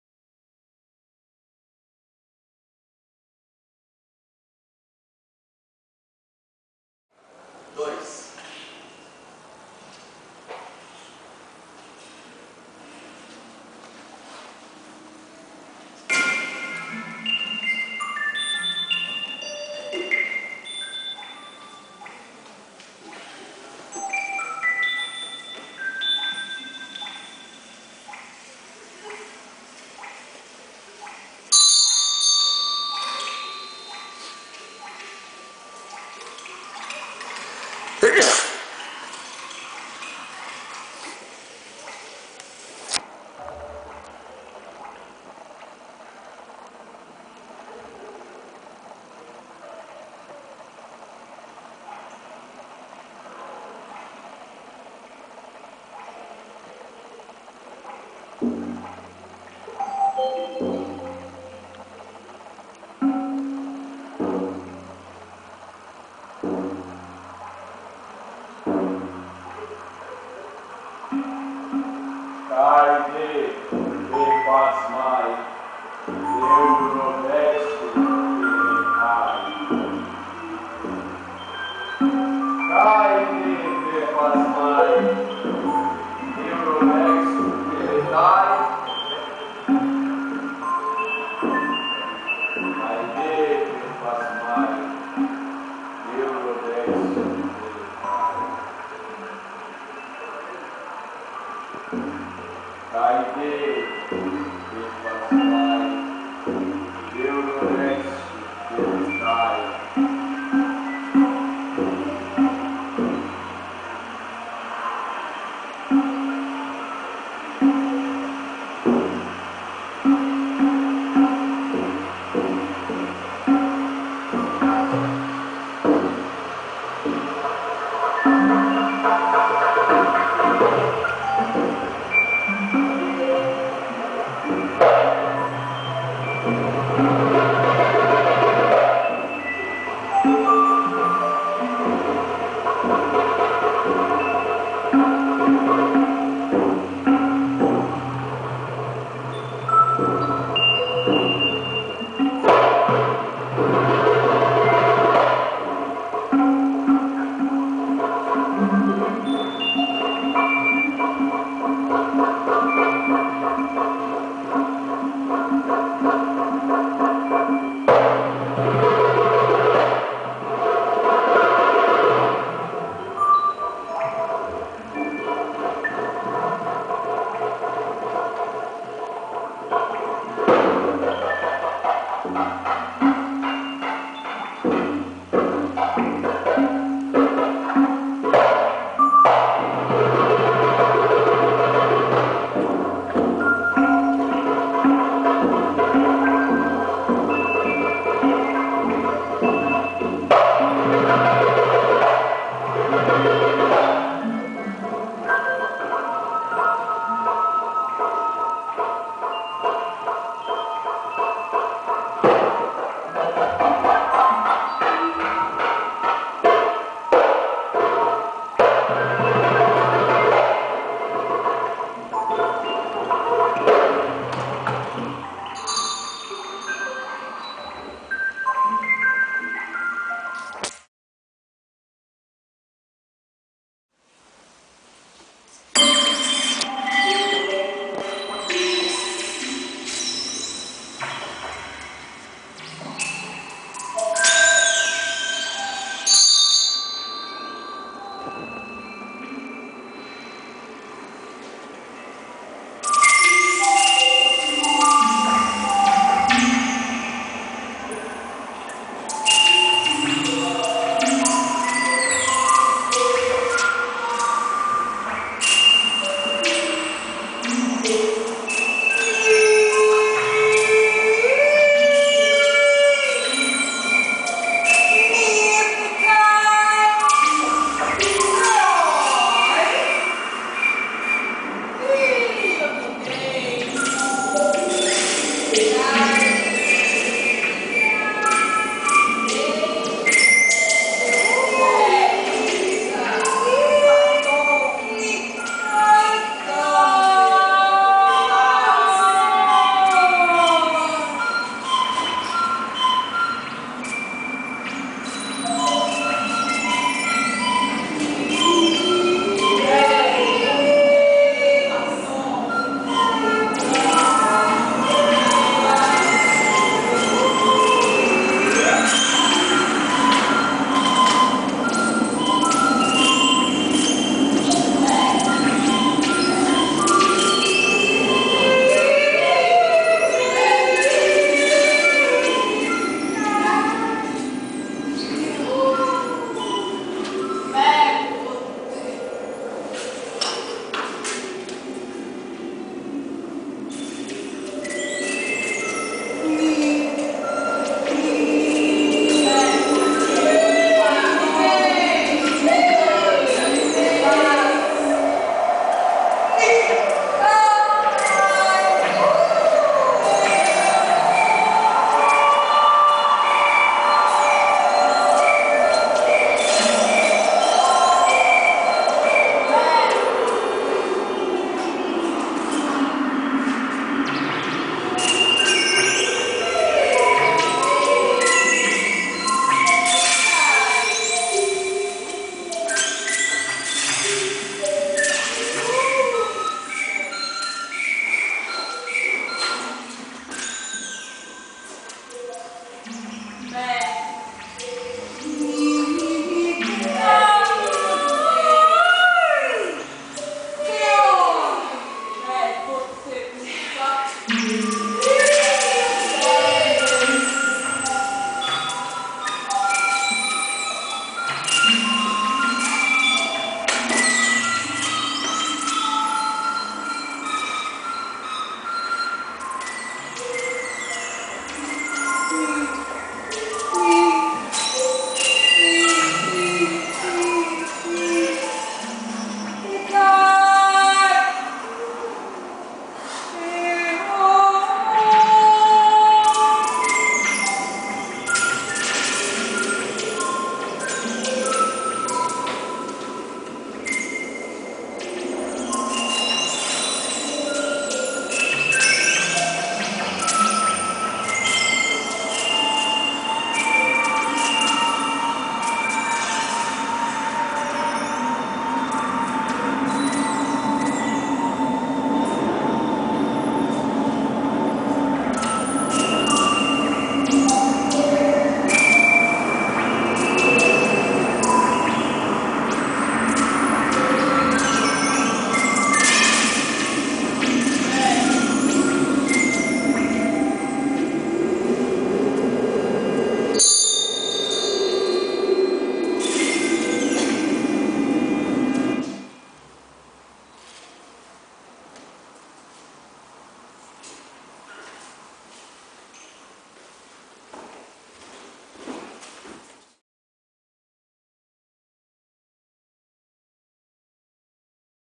Holofractal impromptu #12 on fragments from Aeschylus' The Suppliants - Synolo Iketes 1/3
Synolo Iketes (Suppliants Game).

Performance "Holofractal impromptu #12 on fragments of Aeschylus tragedy The Suppliants" at the 51th Cometa Cenas in Brasília (Brasil), 26/Aug/2010.

Conceived and directed by Eufrasio Prates, with the participation of Laura Muradi, Stephane Paula, Clarice Cabral, Stevan Correa, Mahmud El Masri, Geovani Santoli, Alex Sales, Kai Kundrat, Renata Homem, Cinthia Nepomuceno, supervised by Dr. Marcus Mota (LADI, UnB). FM Synthesizers programmed by John Bischoff.

The work is based on a semiotic improvised dialogue between expressive body movements, ancient Greek fragments from Aeschylus tragedy, percussion, live electronics and fractal synthesizers playing webcam captured movements. This aesthetic interconnection intends to embed many concepts of new physics, like acausality, unpredictability, holonomy and fractality.

Recording thanks to Marcos Fadanelli Ramos.

Part 1/3